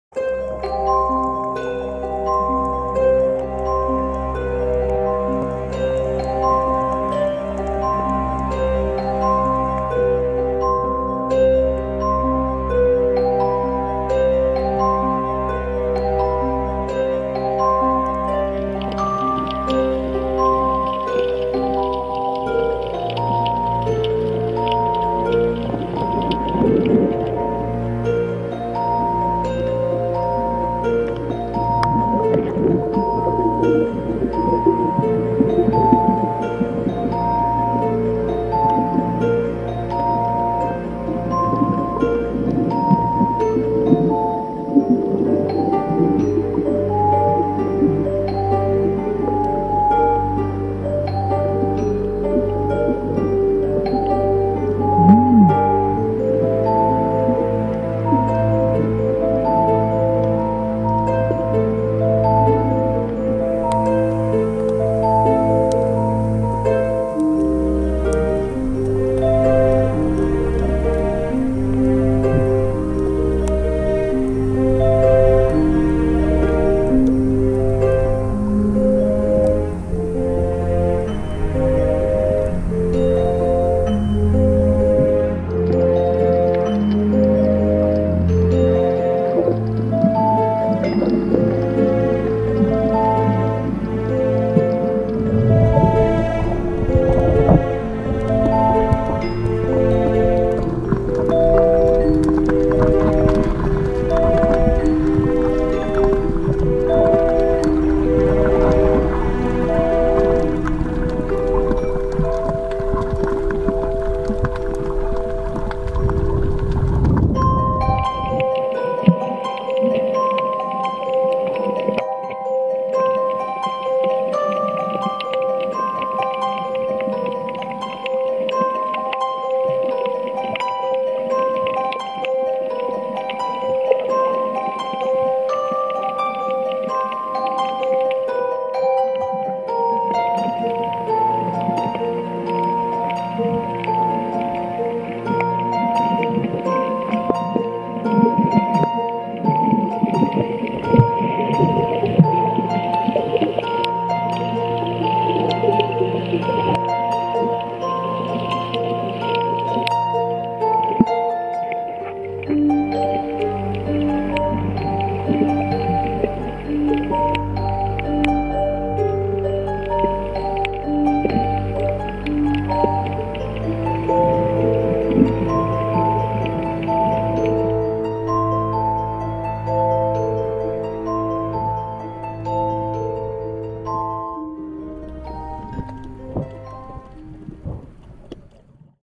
Cuttlefish chameleon
Shelly Ocean Swimmers again discover more Giant cuttlefish at Jibbon Beach. The cuttlefish changed colour to a paler white against the sand and red against the rocks and seaweed.Combined footage from Shelly Ocean Swimmers.